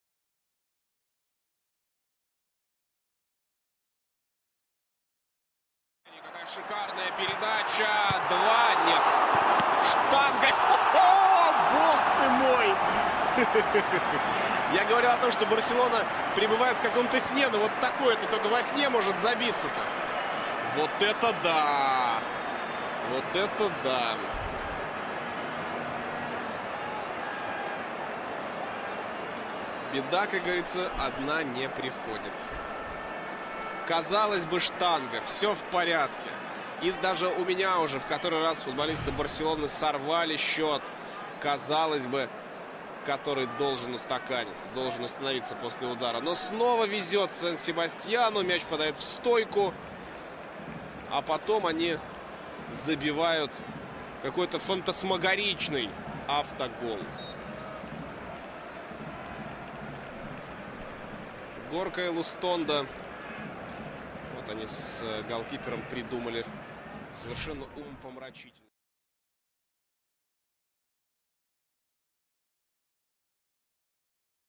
Real Sociedad EPIC OWN GOAL (vs. FC Barcelona | 05/02/2014)
Sociedad's Eñaut Zubikarai scored a bizarre own goal with the help of Gorka Elustondo against Barcelona in the Copa del Rey semi-final first leg.

Ο Ζουμπιακαράι της Σοσιεδάδ πέτυχε ένα σχετικά περίεργο αυτογκόλ με τη βοήθεια του Ελουστόντο εναντίον της Μπαρτσελόνα στον πρώτο αγώνα μεταξύ τους για τα ημιτελικά του Ισπανικού Copa Del Rey.